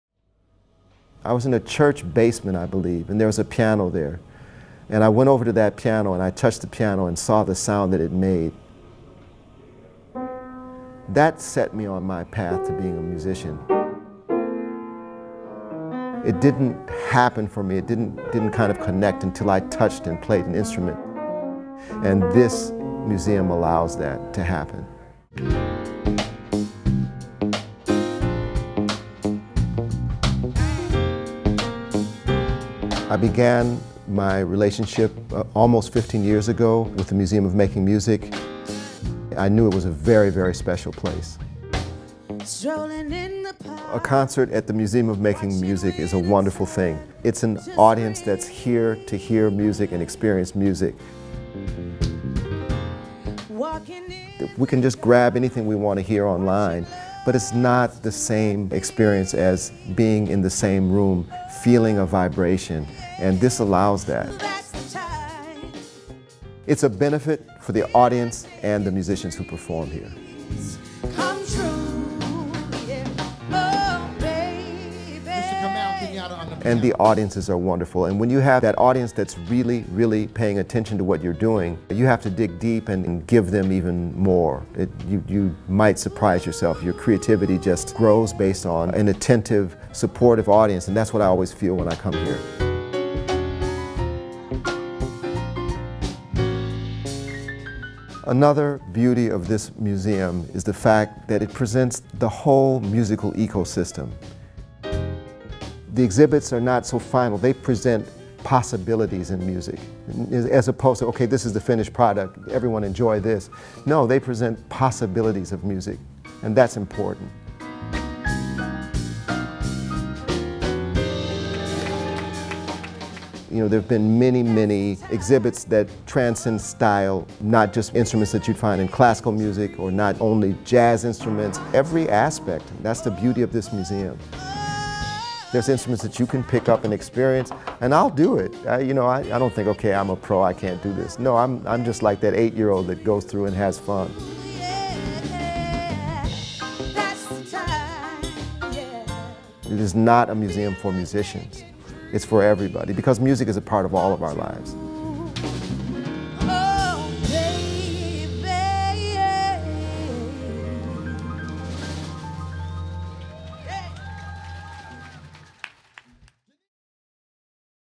Museum of Making Music - Possibilities with Music
San Diego musician Kamau Kenyatta shares an artist's perspective of how the Museum of Making Music allows individual experiences and possibilities with music.

Produced by Enjet Media.